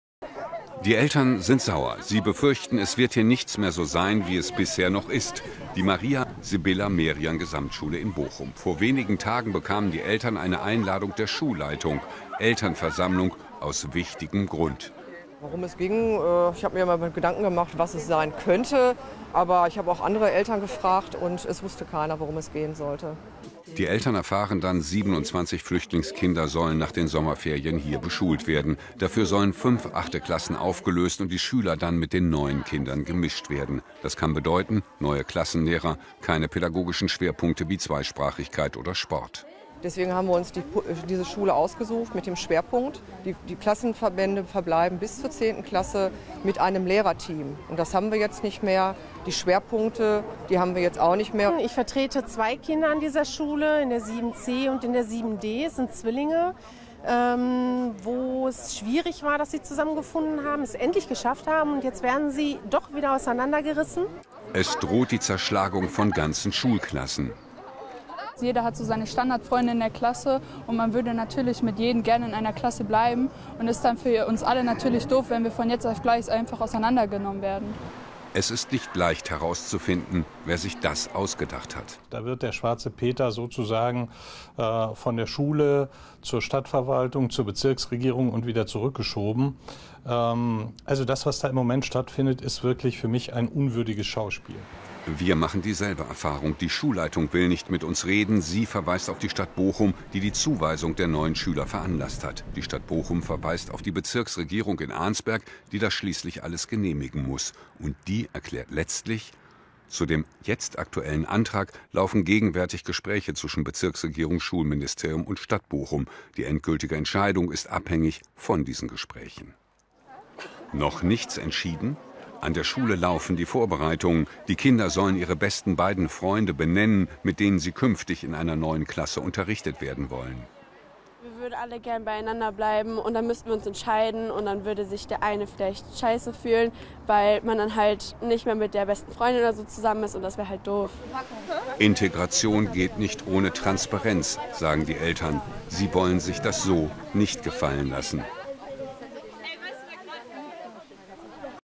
Maria-Sibylla-Merian Gesamtschule in Bochum
12.06.17

Richtig sauer sind viele Eltern an der Maria-Sibylla-Merian Gesamtschule in Bochum-Wattenscheid. Die achten Klassen sollen aufgelöst und neu zusammengemischt werden, damit nach den Sommerferien 27 Flüchtlingskinder integriert werden können. So nicht, sagen die Eltern. Heute soll es an der Schule dazu ein Gespräch mit allen Beteiligten geben. Auch an anderen Schulen gibt es ähnlich Diskussionen.

UPDATE:
Die Eltern haben sich heute mit der Maria-Sibylla-Merian-Schule und der Bezirksregierung getroffen. Das Ergebnis ist, dass die Klassen jetzt so wie vorgesehen neu zusammengestellt werden. Die Alternative wäre gewesen, reine Flüchtlingsklassen einzurichten. Den Eltern war es aber aus sozialen Gesichtspunkten wichtig, dies zu verhindern. Deswegen haben sie den Plänen zähneknirschend zugestimmt.